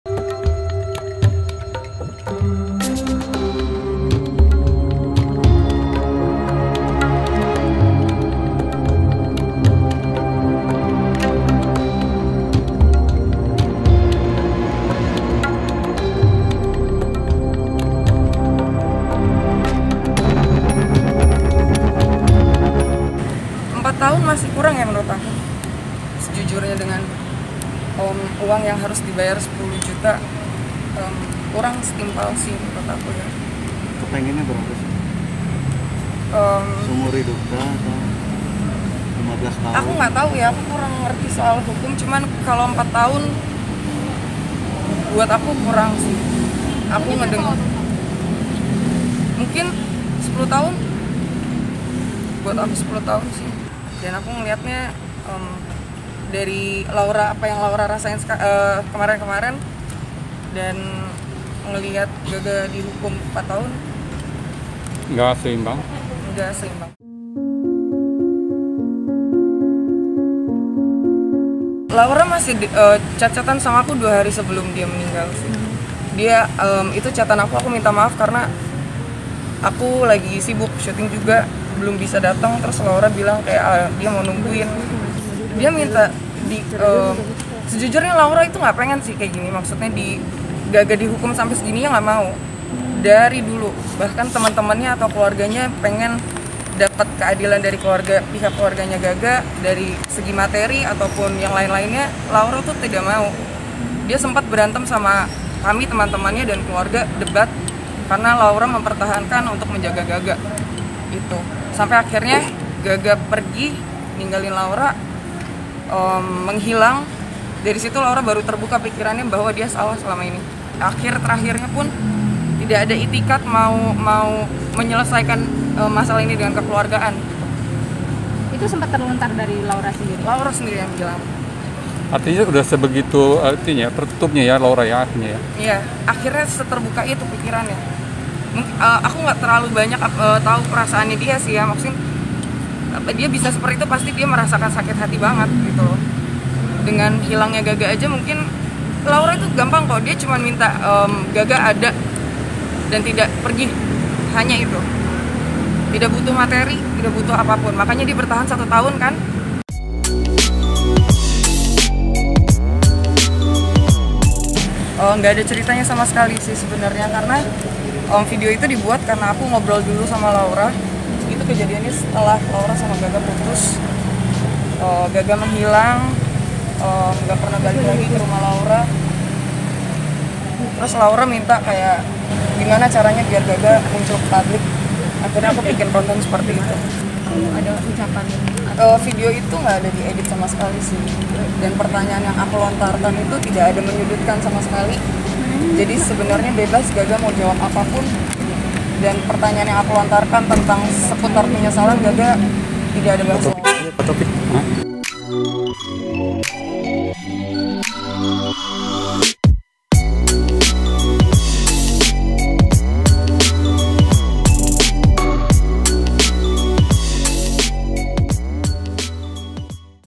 Sahabat Laura Anna Tidak Terima, Gaga Muhammad Hanya Dituntut 4 Tahun 6 Bulan
Sahabat Laura Anna, Erika Carlina merasa tuntutan untuk Gaga Muhammad dalam sidang kasus kecelakaannya di PN Jakarta Timur, Selasa, 4 Januari, tidak sesuai. Kepada wartawan, Erika ungkapkan hal tersebut.